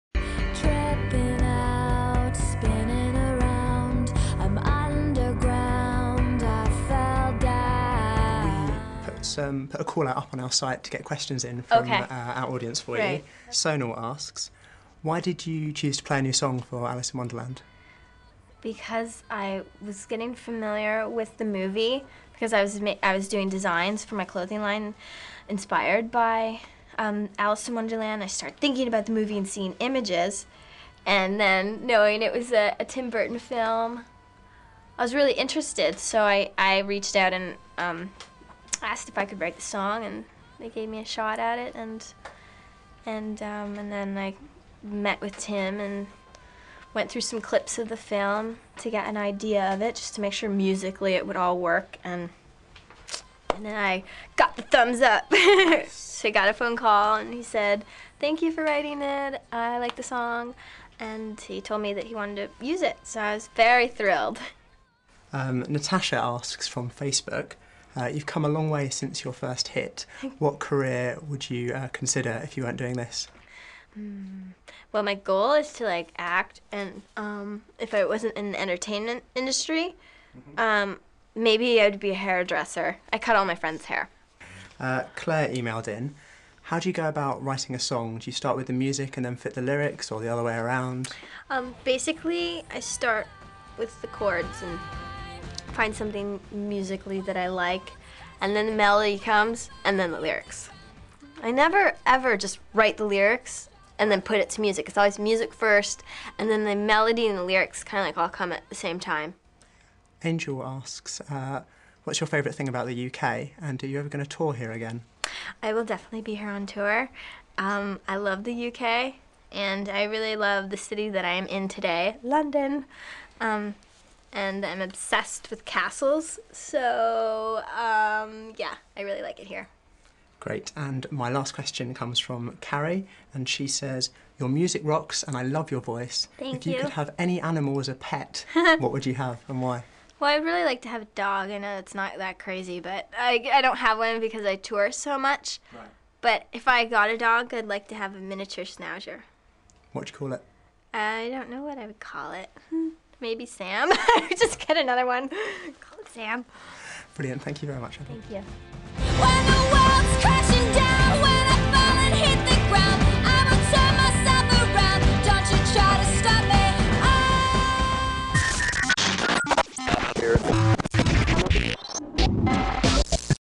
Avril Lavigne - Entrevista sobre Alice in Wonderland
Iconic director Tim Burton has given children's classic Alice In Wonderland a 3D makeover, but even better, he's got Avril Lavigne on songwriting duties for the film. Time for a chat, we reckon.
So, armed with your finest questions, Slink Trev met Avril for a rabbit about the new movie, how she goes about writing songs and crucially, what she'd call her miniature Schnauzer...